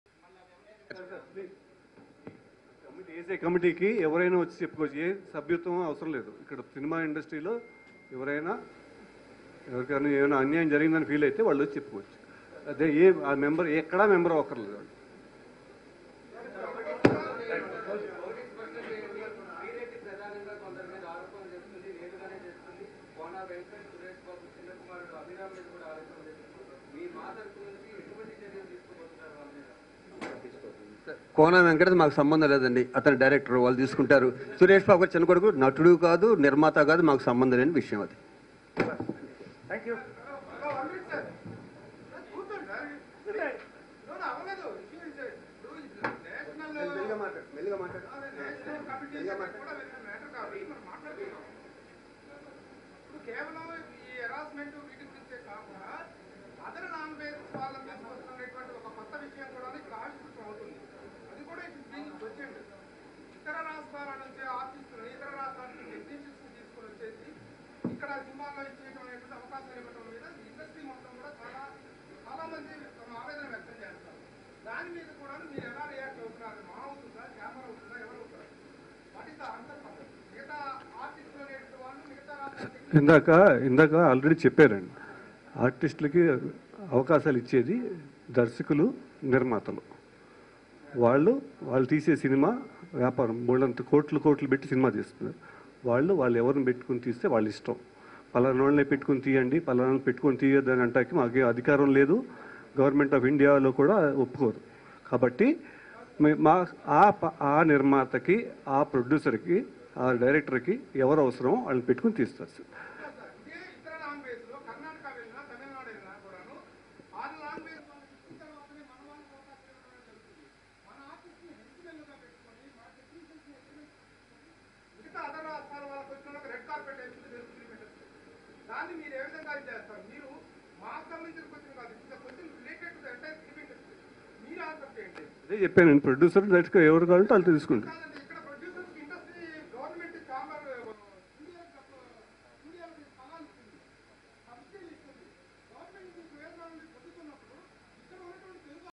కోన వెంకట్ , అభిరామ్ ల గురించి మాకు సంబందం లేదు | Sivaji Raja About Kona Venkat and Abhiram
Actor Sivaji Raja became angry on the issue of Kona Venkat and Abhiram issue with Sri reddy with media people as he told that there is no right for them to talk about them as they are producers and actors, It is their issue to take an artist.